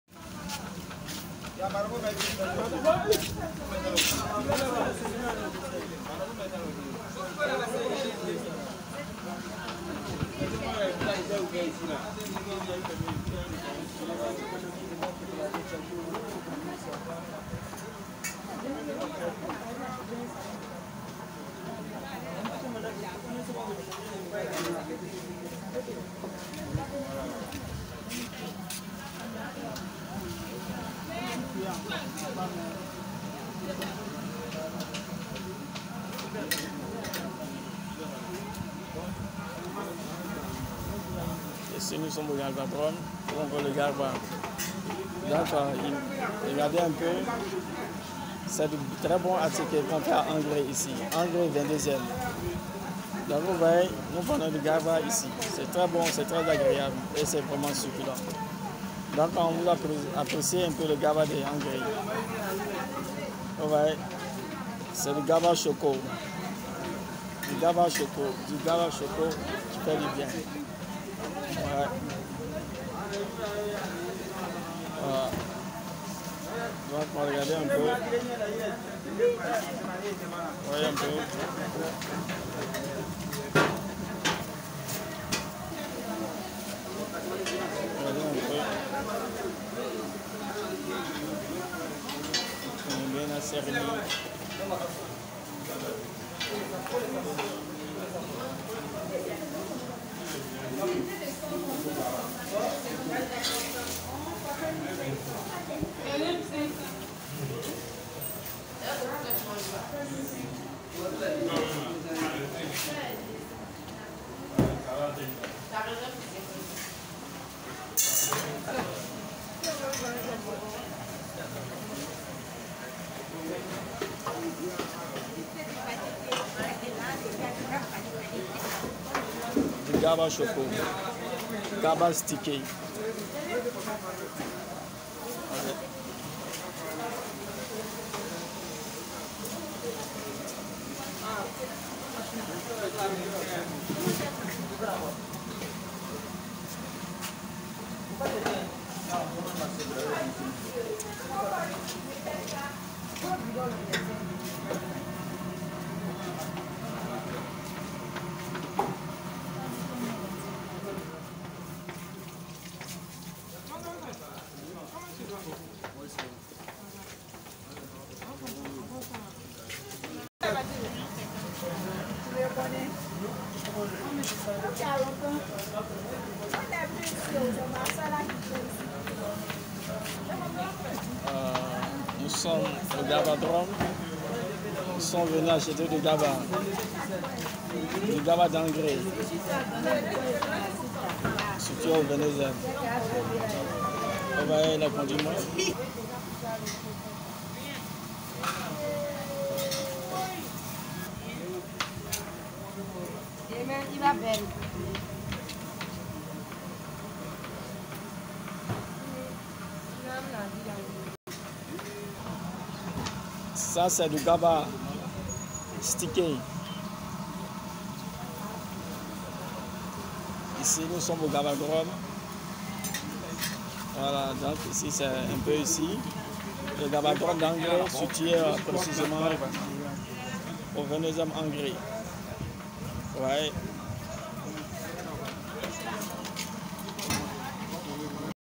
Garba-attieke & thon (Ivoirien )
Gastronomique Ivoirienne, l'attieke au poisson thon grillé, encore appelé ''Garba'' ou ''Zeguen'' s'est imposé comme le Fast-food le plus  populaire en Côte d'Ivoire. Économiquement adaptée à toutes les situations sociales, consistant et surtout pratique, le Garba, essentiellement commercialisé par des expatriés de la sous-région ouest-africaine, incontournable en dépit de tout, est de plus en plus exploré par de jeunes entrepreneurs, parmi lesquels des Ivoiriens.